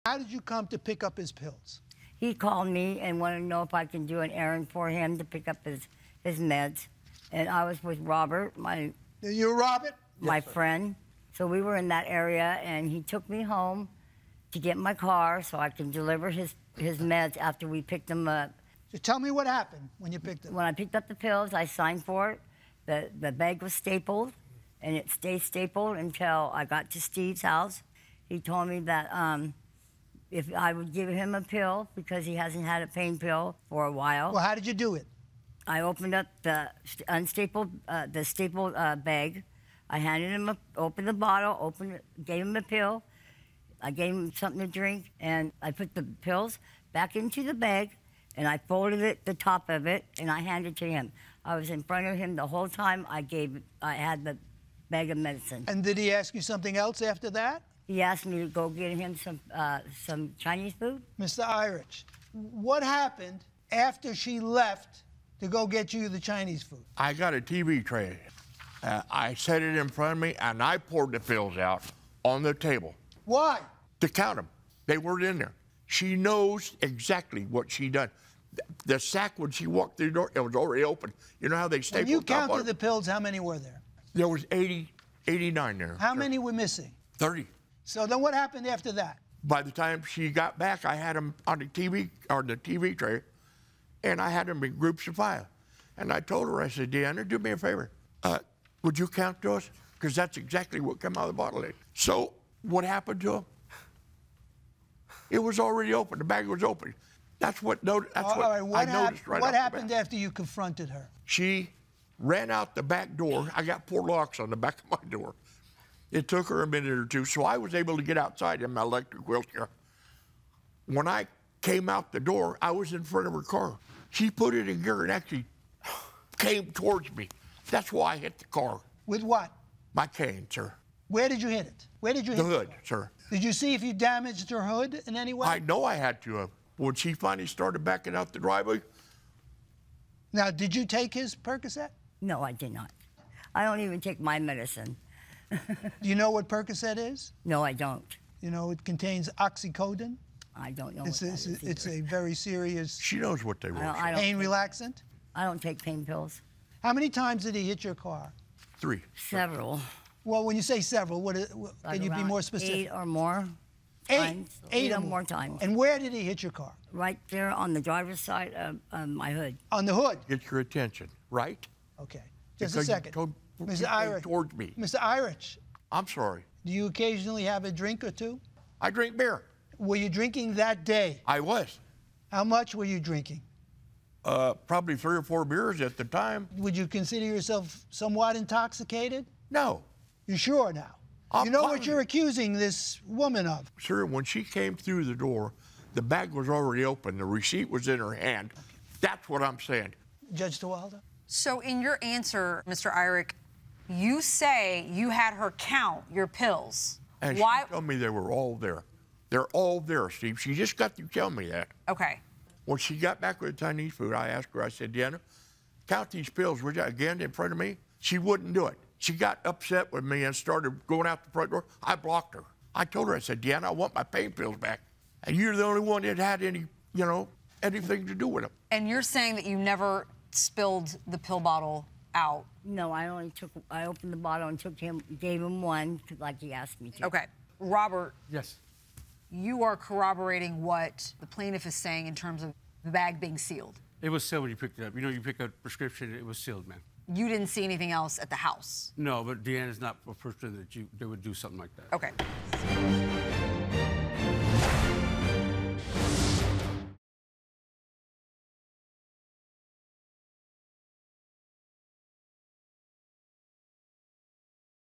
Helper Claims Elderly Client Held Her Hostage! | Part 1
Deeann claims Steve accused her of stealing his medication, but the pharmacy bag was stapled shut!